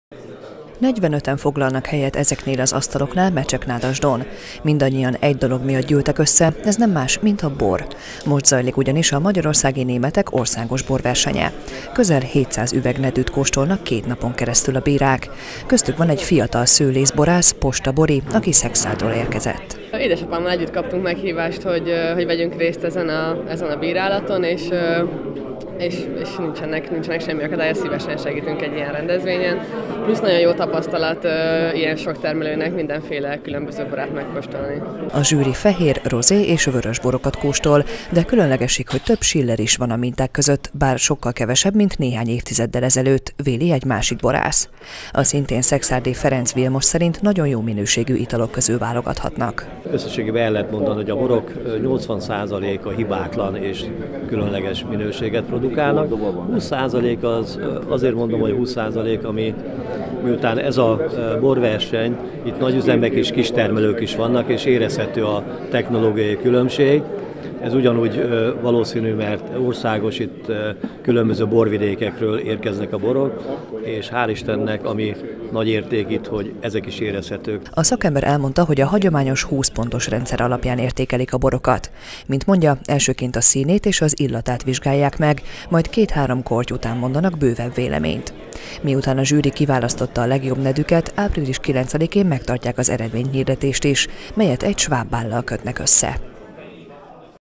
Magyarországi Németek Országos Borversenye
Közel 700 üveg bort neveztek az idei Magyarországi Németek Országos Borversenyére. A megmérettetést Mecseknádasdon rendezik, ahol most zajlik a zsűrizés. 45 szőlész-borász értékeli a fehér-, rozé- és vörösborokat. Ebben az évben különlegesség, hogy több siller is van a minták között, ami manapság ritkaságnak számít. Az eredményhirdetést április 9-én tartják, egy sváb bállal egybekötve.